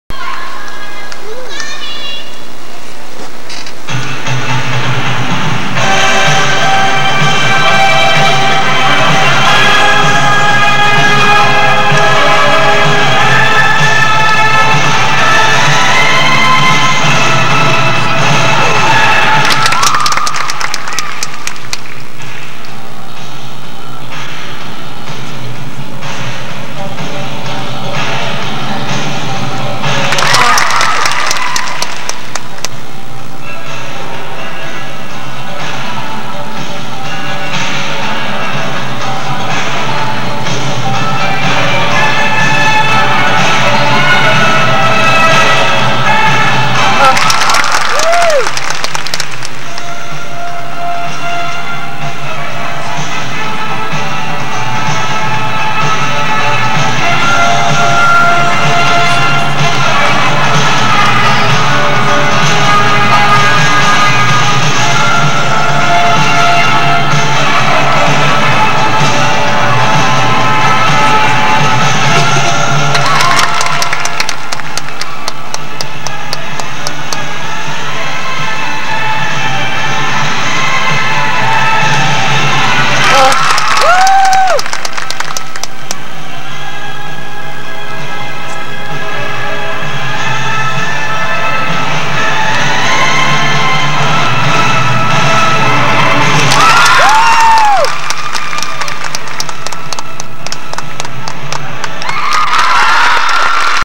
1988 USTA National Championships, Reno, Dance Twirl FINALS, Bonnie Palacious
Reno, NV